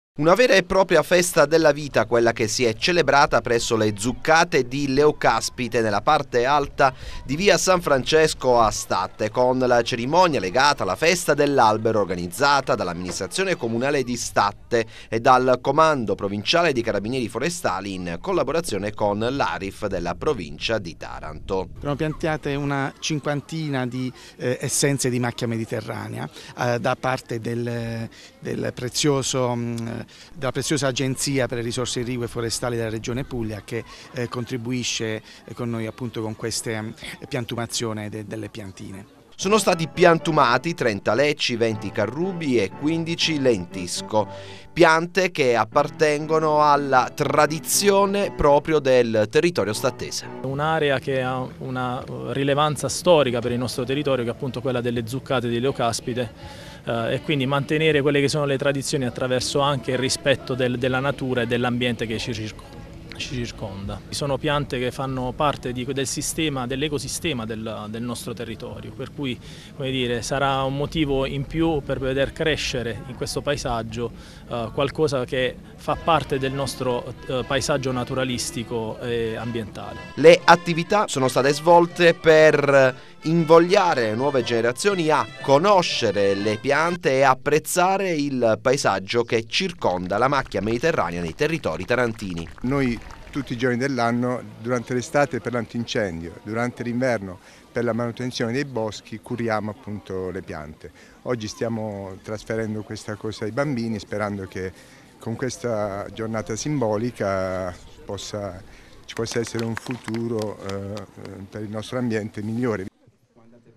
UN ALBERO DI SPERANZA di Francesco Friuli
Si è celebrata a Statte la giornata nazionale degli alberi.